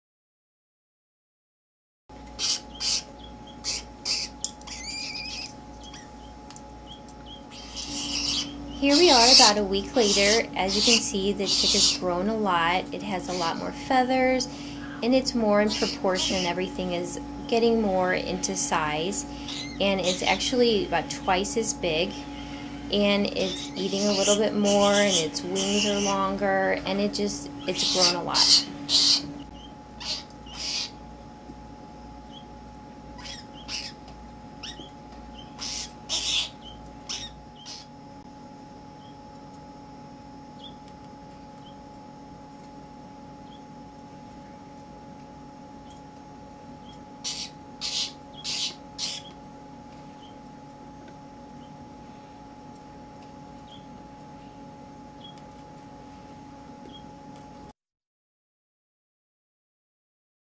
Snowy egret chick (21 days old) at Saint Louis Zoo
The snowy egret chick is now 21 days old. It has about doubled its size in just one week. Its starting to look more and more like its parents every day!

The chick is being hand reared by bird keepers in an off display area of the Bird House.  The diet consists of big bites of fish and mice along with some added vitamins.